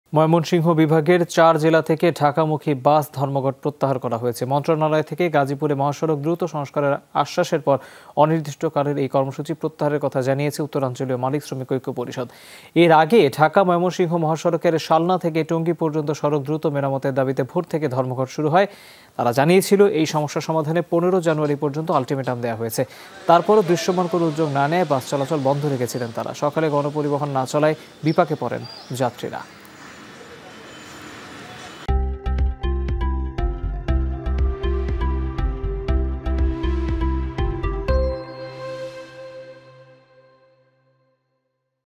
ময়মনসিংহের পরিবহন ধর্মঘট প্রত্যাহার | Bus Strike
ময়মনসিংহ বিভাগের চার জেলা থেকে ঢাকামুখী বাস ধর্মঘট প্রত্যাহার হয়েছে।